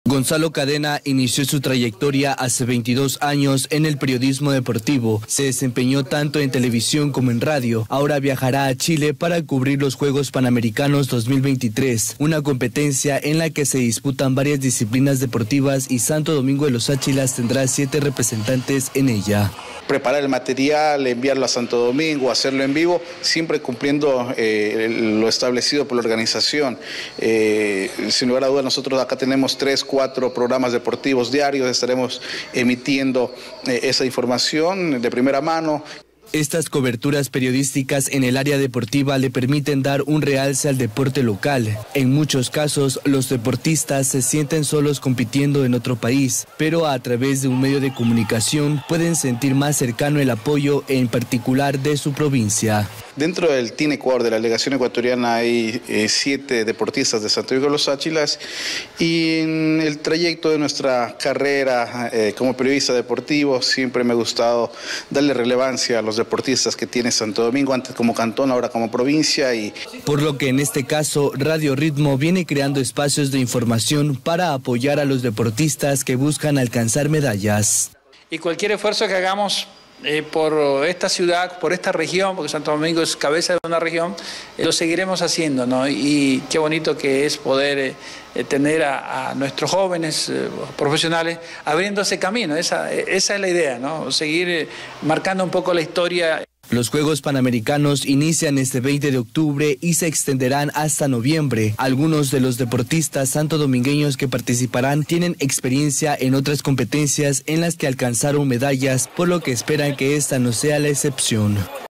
El periodismo deportivo es una rama de la comunicación sin explotar en Santo Domingo
El periodismo deportivo es una rama de la comunicación no muy explotada en Santo Domingo de los Tsáchilas. Son muy pocos los profesionales locales que se dedican a compartir las vivencias del deporte en esta provincia. Uno de ellos, es Gonzalo Cadena, quien por cuarta vez saldrá del país a una cobertura internacional: los juegos Panamericanos Chile 2023.